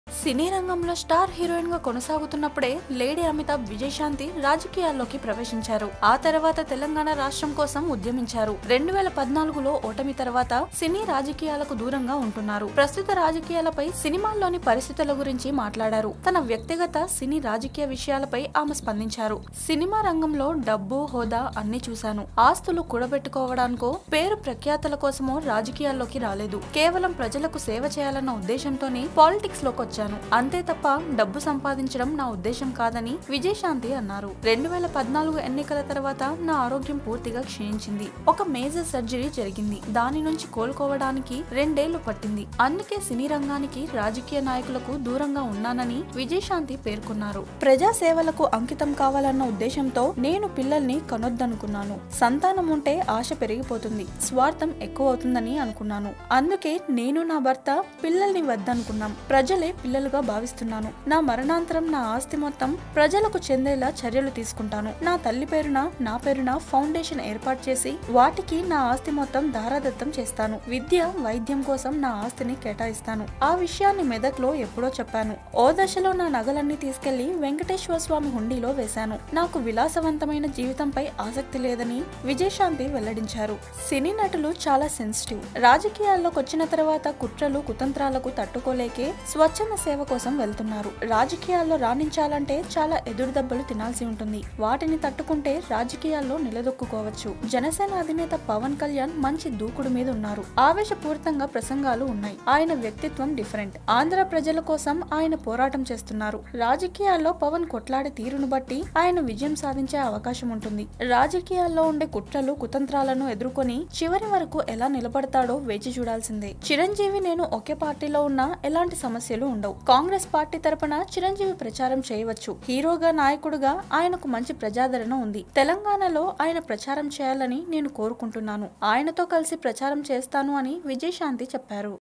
Pawan Kalyan Rocks In Politics : Vijayashanthi | Filmibeat Telugu
Actor, Politician Vijayashanthi responded on contemporary Issue in the society. She speaks about her personal life and Political journey. For the sake of Public service, She took decision for not to give birth to Kids. Vijayashanthi welcomes Pawal Kalyan into politics.

సినీ రంగంలో స్టార్ హీరోయిన్‌గా కొనసాగుతున్నప్పుడే లేడీ అమితాబ్ విజయశాంతి రాజకీయాల్లోకి ప్రవేశించారు. ఆ తర్వాత తెలంగాణ ప్రత్యేక రాష్ట్రం కోసం ఉద్యమించారు. 2014లో ఓటమి తర్వాత సినీ, రాజకీయాలకు దూరంగా ఉంటున్నారు. ప్రస్తుత రాజకీయాలపై, సినిమాలోని పరిస్థితుల గురించి మాట్లాడారు. తన వ్యక్తిగత, సినీ, రాజకీయ విషయాలపై ఆమె స్పందించారు. ఆమే ఏమన్నారంటే..